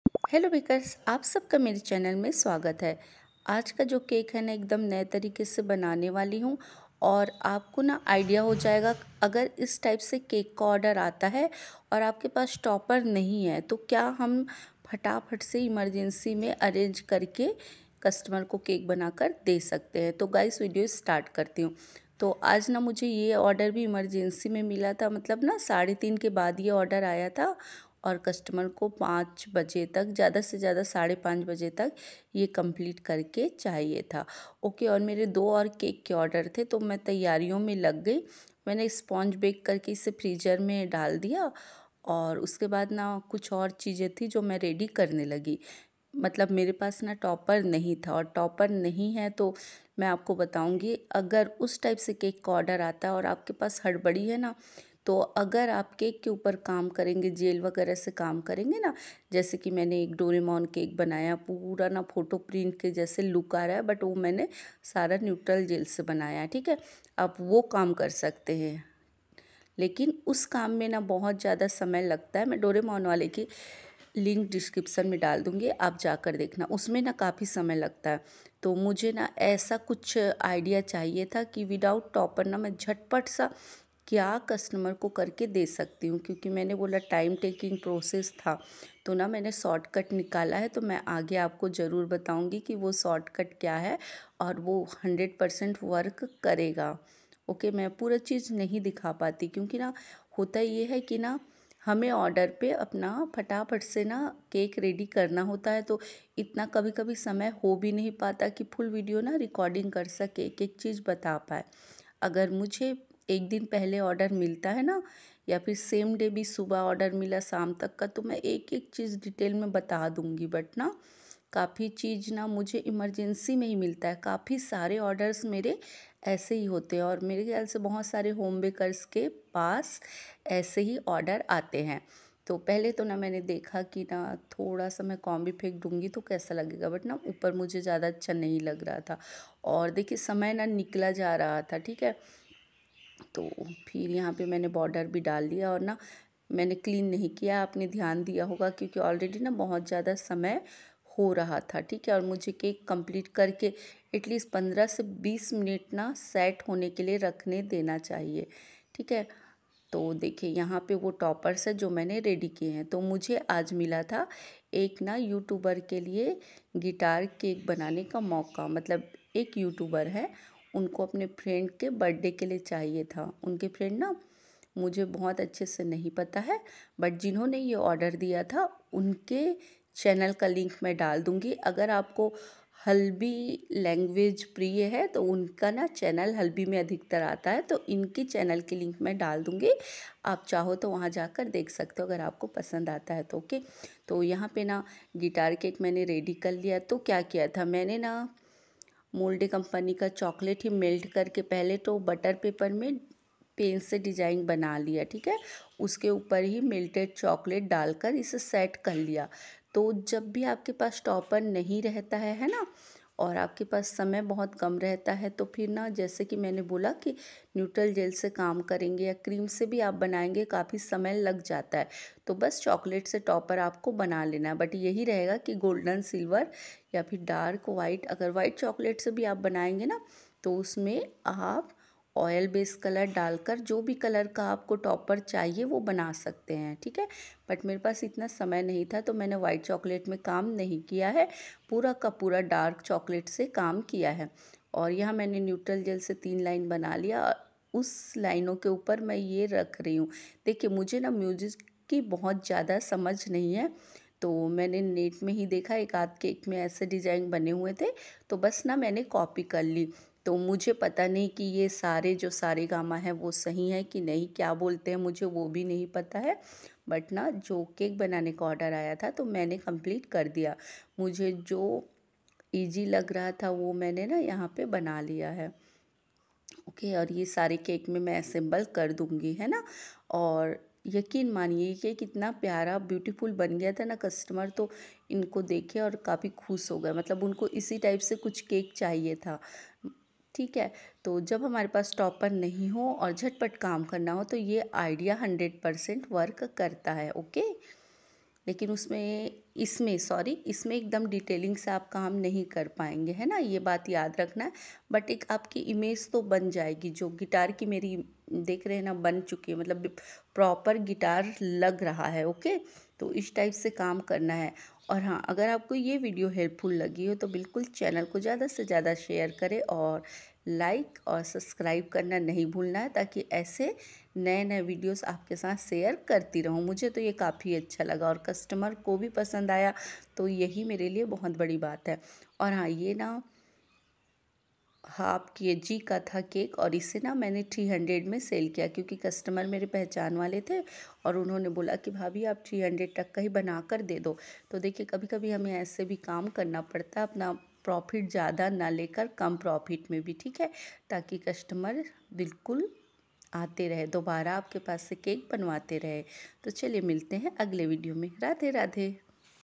how to make Guitar cake ! घर में Guitar topper chocolate se कैसे तैयार किया?
how to make Guitar cake ! घर में Guitar टापर कैसे तैयार किया punamscookingandbaking costomized cake design#urgent order's#chocklate

How to make chocolate guitar topper at home easy